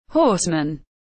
How to pronounce Horseman / Horseman pronunciation
Raccoon teaches you how to pronounce Horseman . Raccoon vous apprends a prononcer Horseman . Como pronunciar Horseman